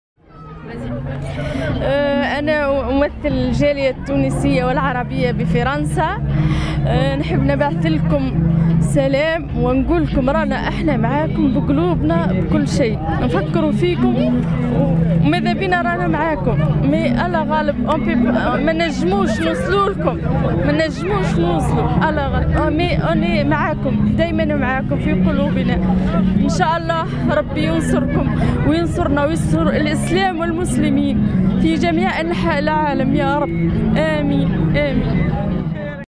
NAZIHA's message to support civilians in Gaza 2014
People in the streets of Marseille, France, lend their moral support to the civilians who are victims of Israël's military operation in Gaza Strip since the beginning of July 2014.